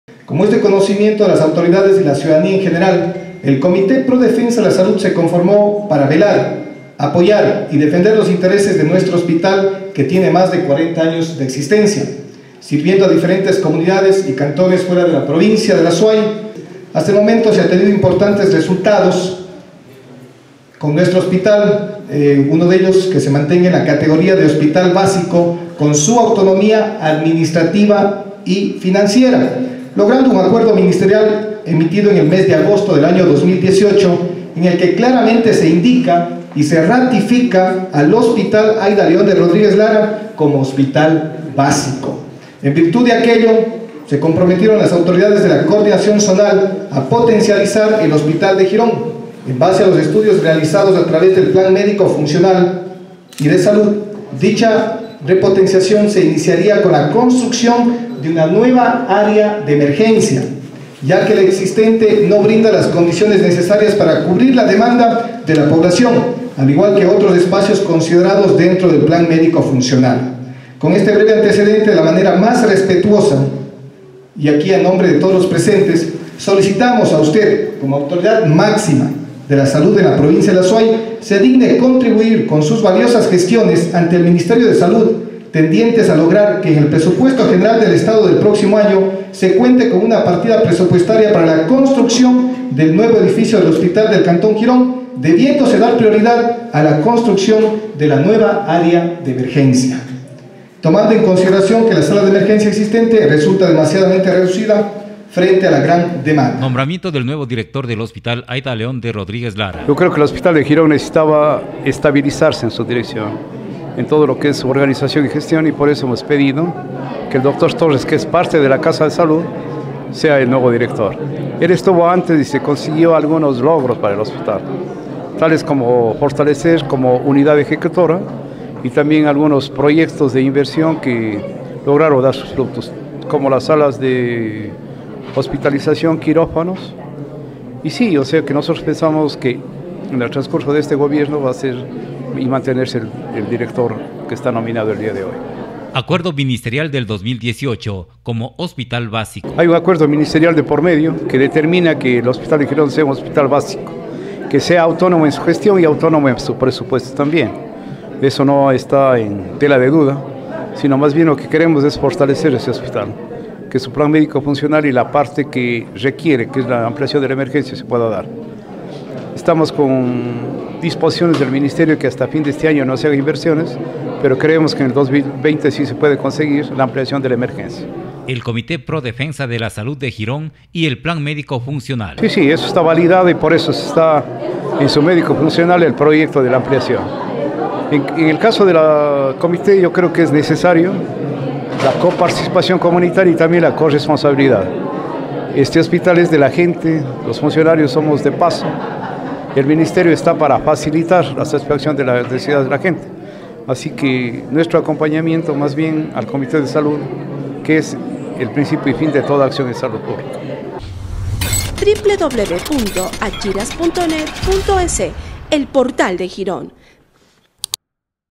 Gironenses hicieron petición al Director Zonal 6 de Salud para mejorar el hospital
Los integrantes del Comité Pro Defensa de la Salud de Girón, durante el acto de posesión del director del hospital “Aida León de Rodríguez Lara” entregaron un oficio al director de la Zonal 6 de Salud, Dr. Julio Molina, en el cual manifiestan la razón de su conformación y, solicitaron se considere el Plan Médico Funcional y la urgencia de adecuar la sala de emergencias. La autoridad de salud se refirió a los temas y dio esperanzas.